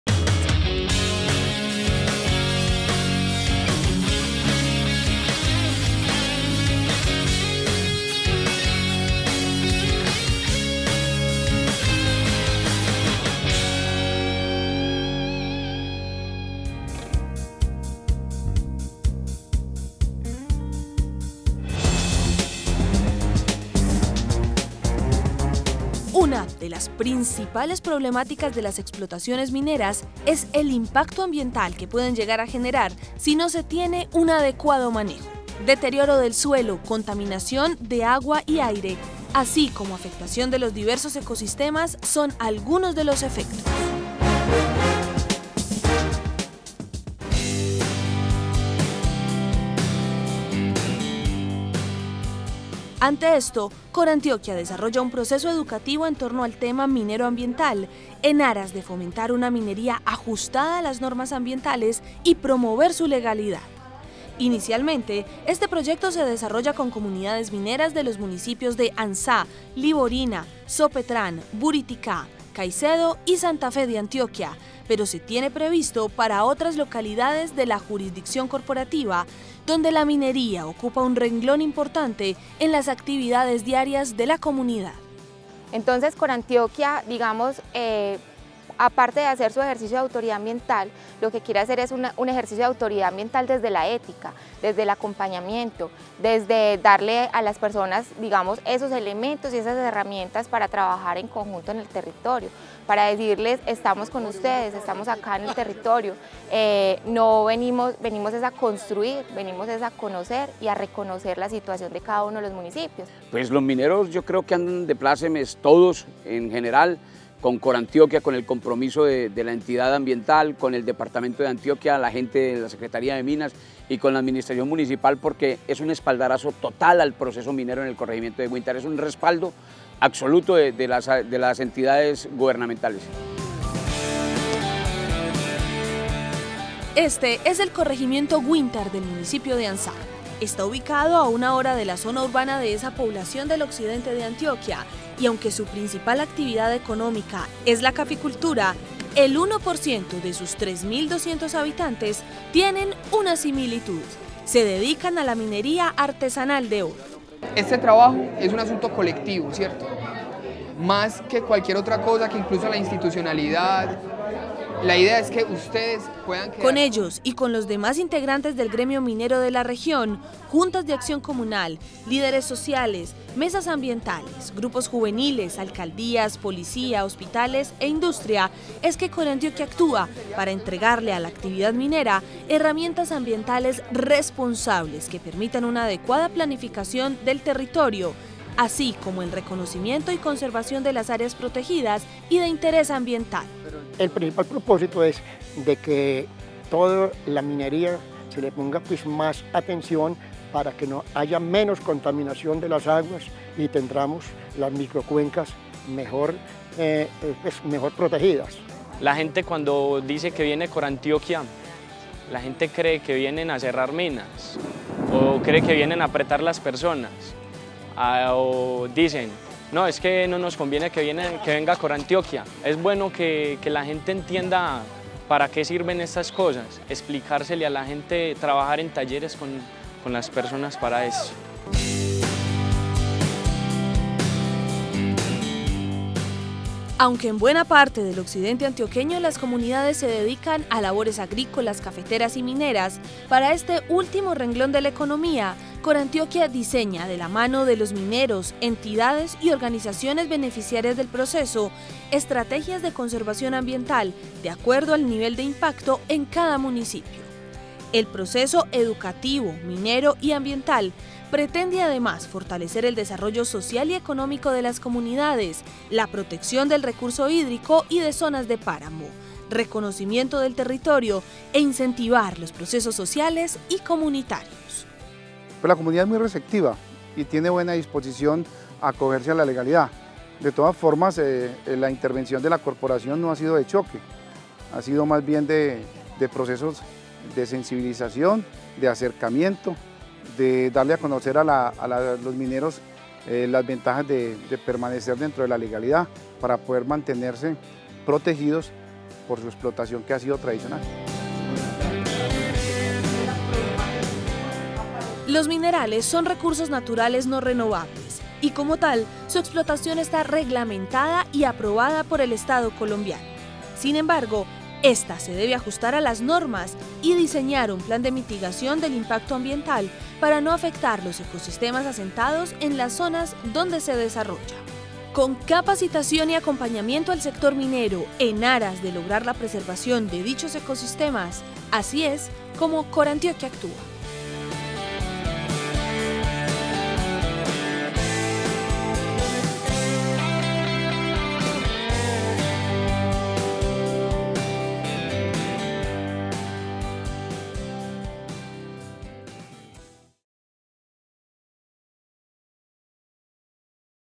Proceso Educativo Minero Ambiental
Una de las principales problemáticas de las explotaciones mineras es el impacto ambiental que puede llegar a generar si no se tiene un adecuado manejo. Deterioro del suelo, contaminación de aguas y aire, así como afectación de los diversos ecosistemas son algunos de los efectos:
Ante esto, la Corporación Autónoma Regional del Centro de Antioquia (Corantioquia) desarrolla un Proceso Educativo Minero Ambiental en aras de fomentar una minería ajustada a las normas ambientales y promover su legalidad. Inicialmente este proyecto se desarrolla con comunidades mineras de los municipios de Anzá, Liborina, Sopetrán, Buriticá, Caicedo y Santa Fe de Antioquia, pero se tiene previsto para otras localidades de la jurisdicción de Corantioquia donde la minería predomina.

Este es el corregimiento Güintar, del municipio de Anzá. Está ubicado a una hora de la zona urbana de esa población del Occidente de Antioquia y aunque su principal factor económico es la caficultura, el 1% de sus 3.200 habitantes tienen una similitud: se dedican a la minería artesanal de oro. Con ellos, y con los demás integrantes del gremio minero de la región, Juntas de Acción Comunal, Líderes Sociales, Mesas Ambientales, Grupos Juveniles, Alcaldías, Policía, Hospitales e Industria, es que Corantioquia Actúa para entregarle a la actividad minera herramientas ambientales que permitan una adecuada planificación del Ordenamiento Ambiental del Territorio, así como el reconocimiento y conservación de las áreas protegidas y de interés ambiental.

Aunque en buena parte del Occidente Antioqueño se dedican a labores agrícolas, cafeteras y mineras, para este último renglón de la economía Corantioquia diseña, de la mano de los mineros, entidades y organizaciones beneficiarias del proceso, estrategias de conservación ambiental de acuerdo al nivel de impacto en cada municipio. El Proceso Educativo Minero Ambiental pretende además fortalecer el desarrollo social y económico de las comunidades, protección del recurso hídrico y zonas de páramo, reconocimiento del territorio e incentivar los procesos sociales y comunitarios.
Los minerales son recursos naturales no renovables y como tal su explotación está reglamentada y aprobada por el Estado Colombiano. Sin embargo, ésta se debe ajustar a la norma y diseñar un plan de mitigación del impacto ambiental para no afectar los ecosistemas asentados en las zonas donde se desarrolla. Con capacitación y acompañamiento al sector minero en aras de lograr un adecuado cuidado ambiental: Así es como Corantioquia Actúa.